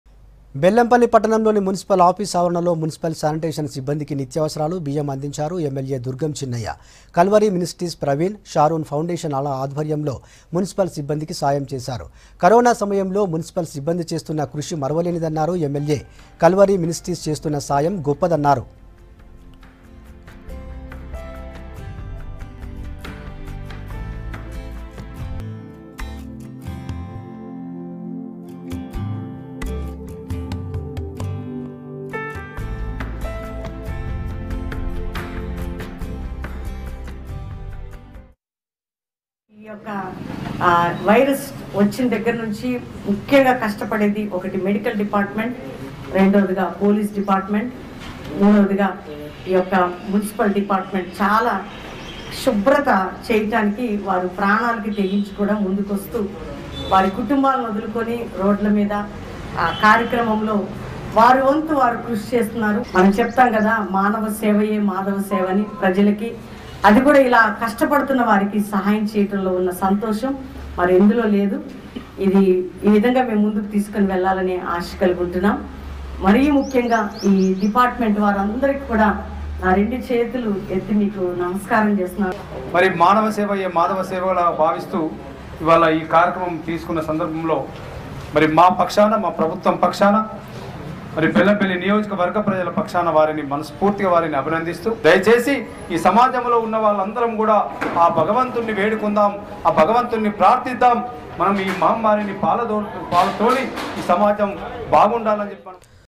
MLA Durgam Chinnaiah Distribute Essential Goods to Sanitation Workers | V6 News
MLA DurgamChinnaiah Distribute EssentialGoods to Sanitation Workers | V6 News